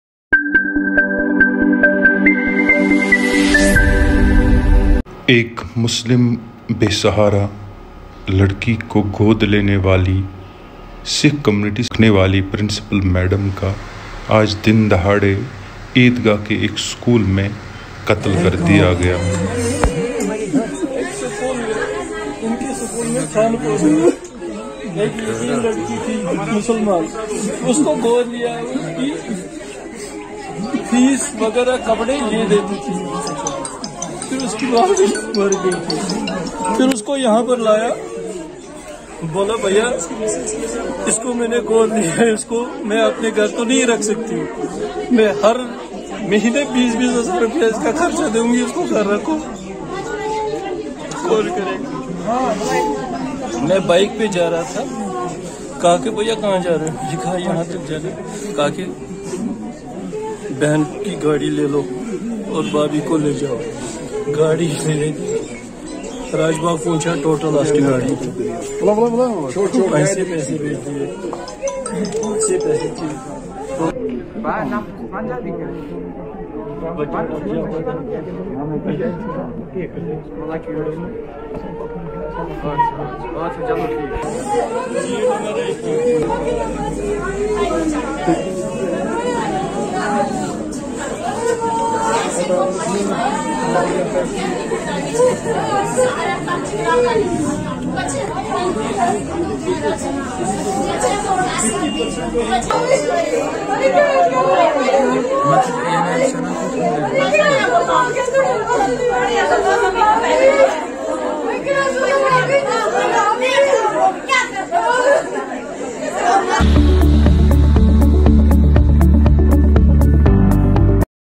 Who Is Going To Take Care Of The Muslim Girl Who Was Adopted By The Principle Who Got Killed?
Watch This Emotional Story To Know:

Who Is Going To Take Care Of The Muslim Girl Who Was Adopted By The Principal Who Got Killed?